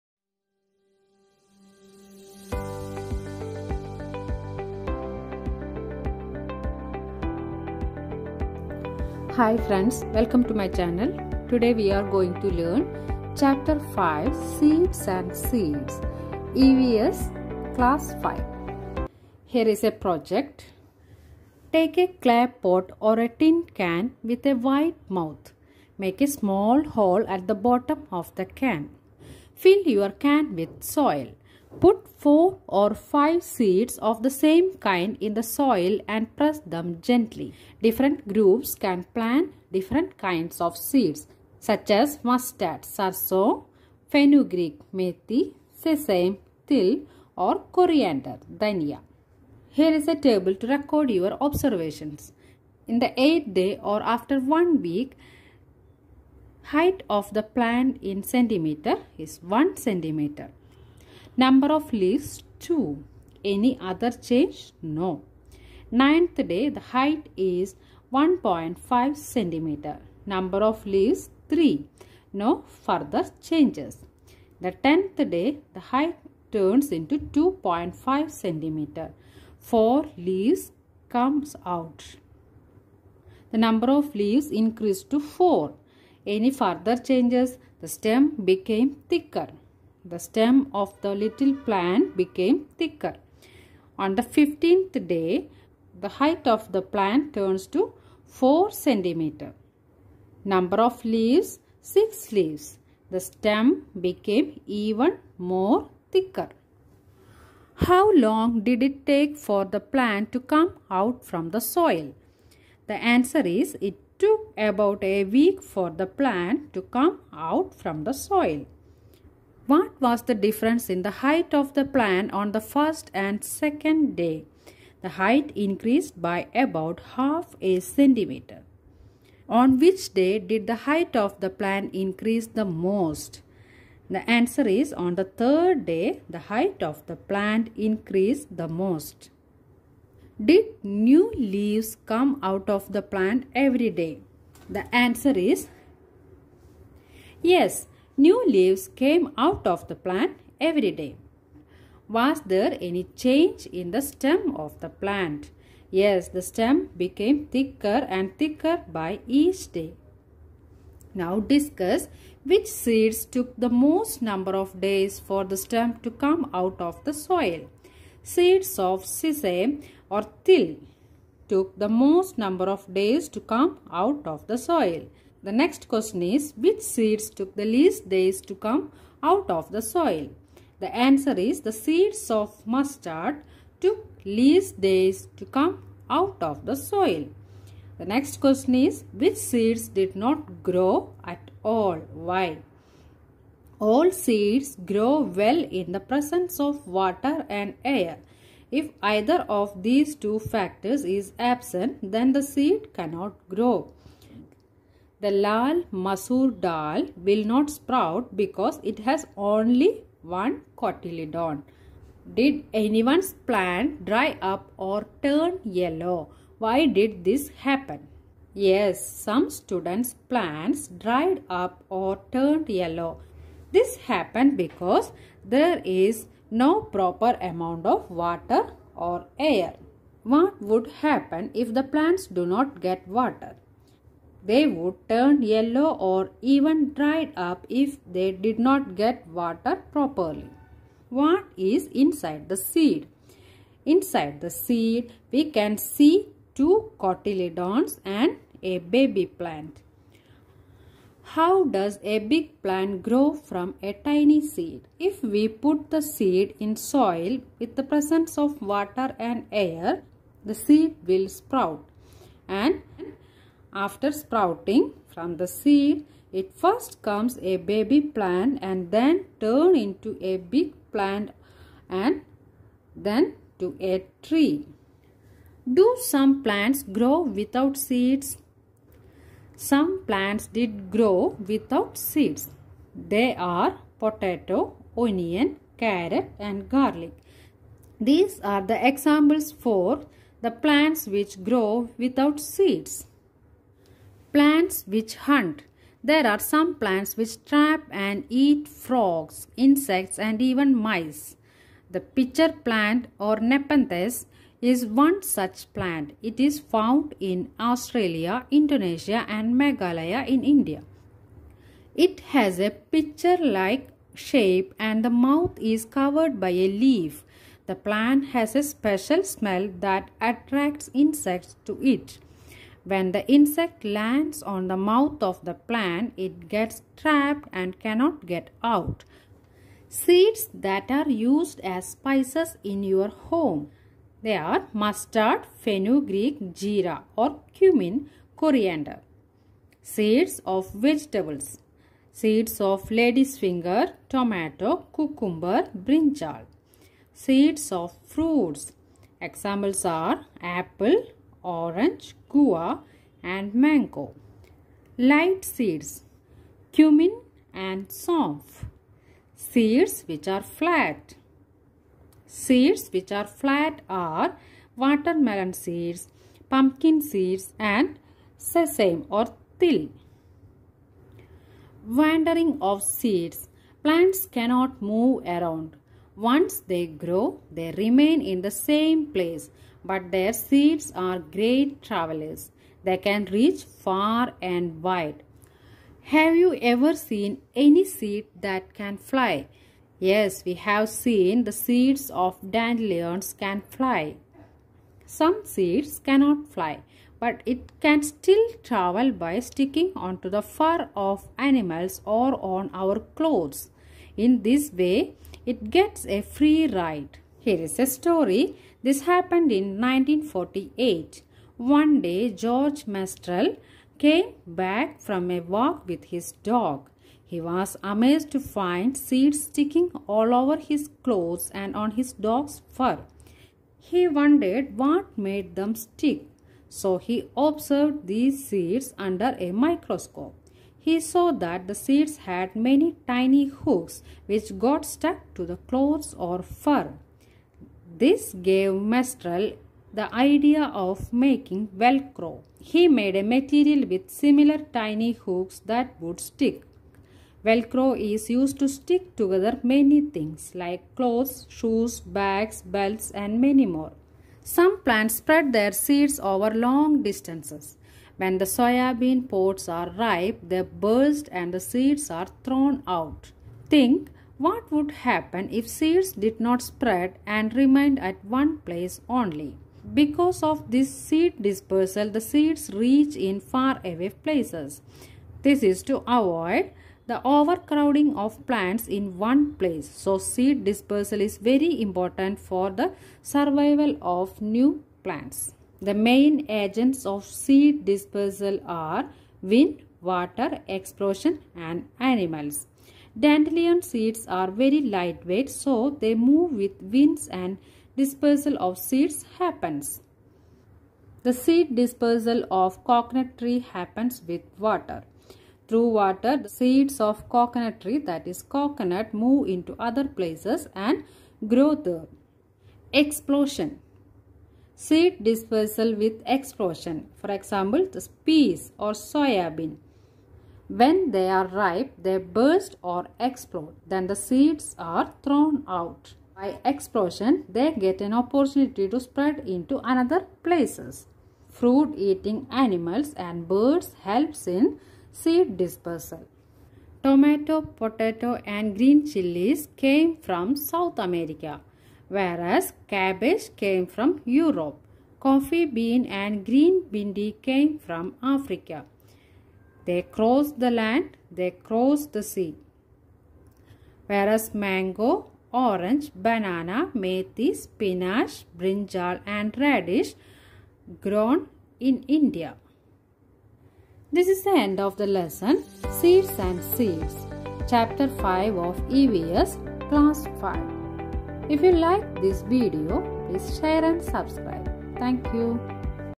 SEEDS AND SEEDS | CLASS 5 EVS CHAPTER 5 | NCERT | PART - 2
Seeds and Seeds , chapter 5 of Environmental studies of class 5. In this part we  are discussing about ,how much time did a seed take to sprout and to grow as a plant. Different kinds of seeds  and the different ways of seed dispersal. We also studies about the various agents of seed dispersal and the significance of that in nature.